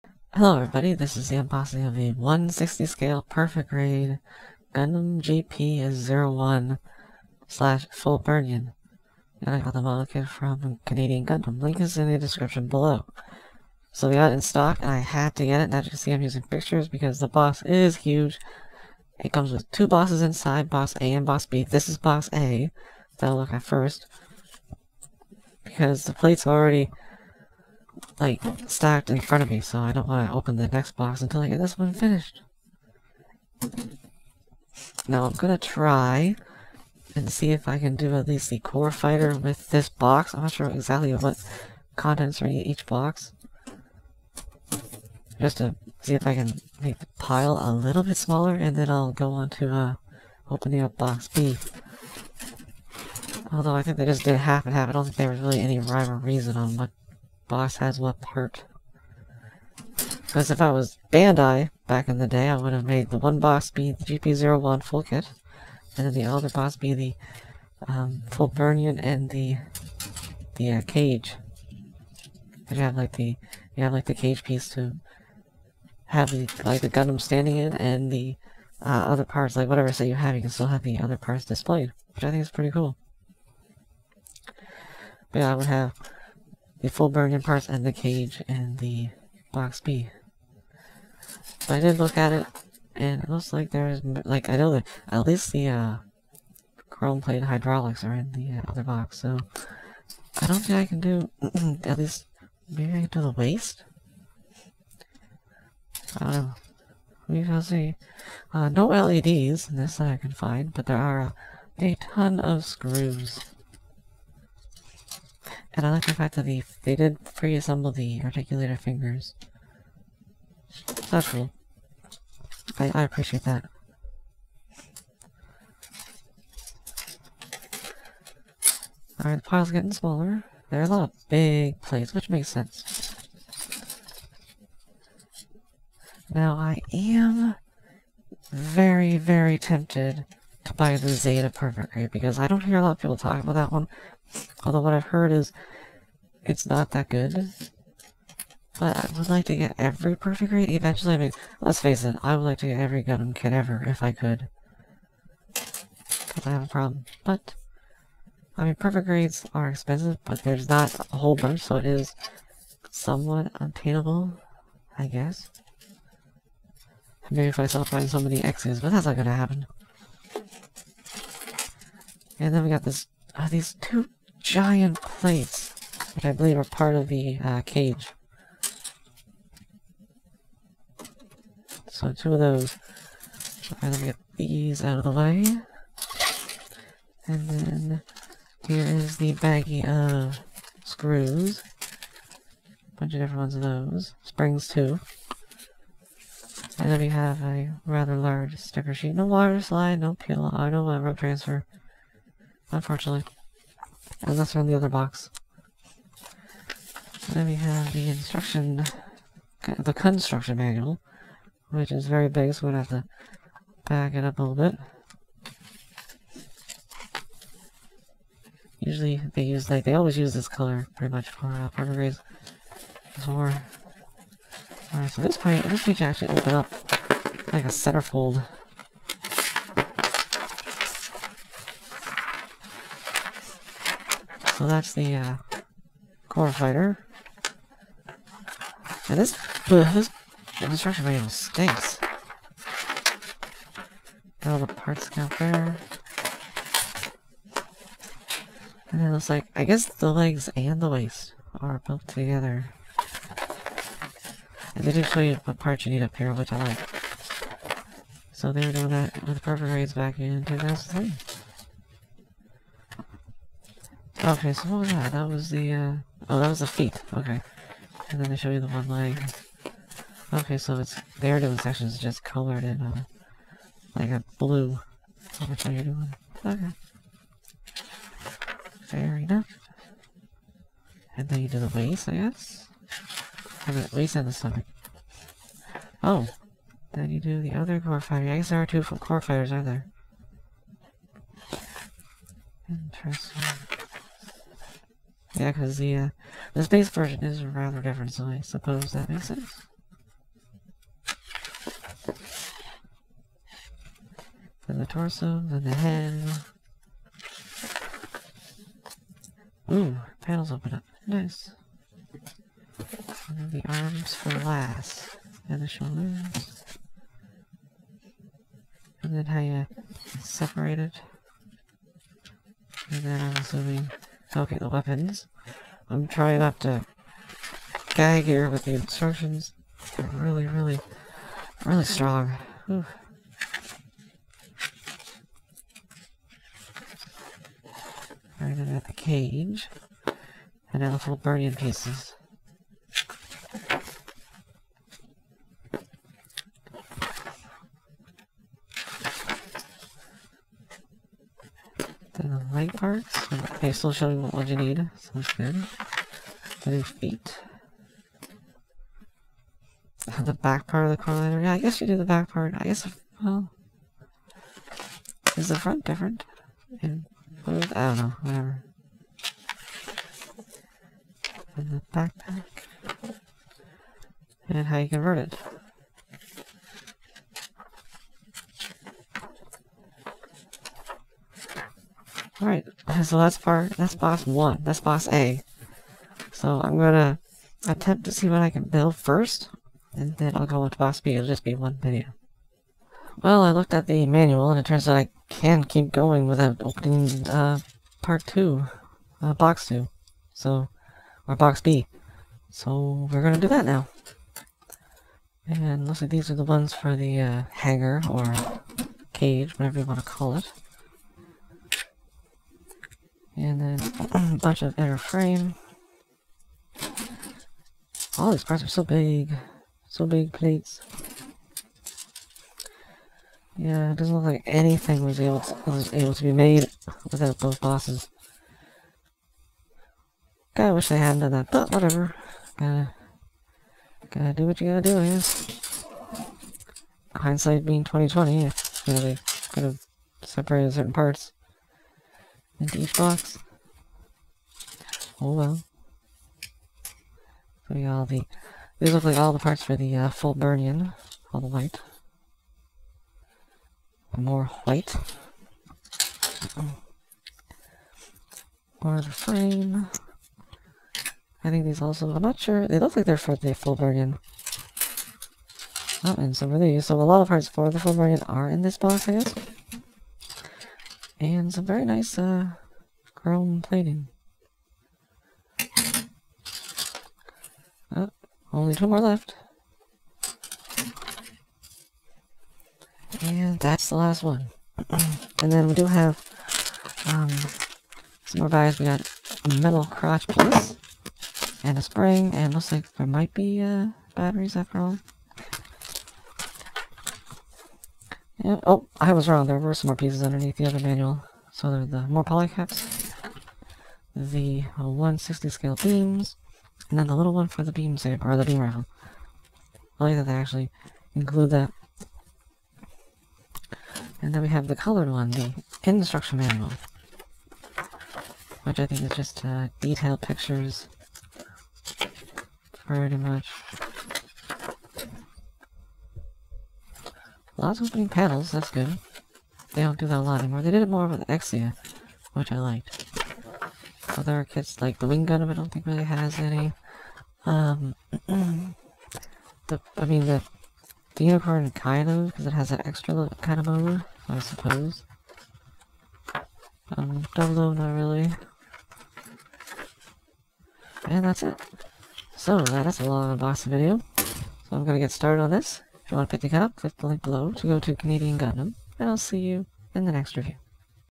Unboxing: 1/60 PG Gundam GP01/Fb
This kit is so big it comes in two seperate boxes inside the box. I just wish they had kept the base Gundam in one box, and the space armor with the hanger in another. Less of a mess that way.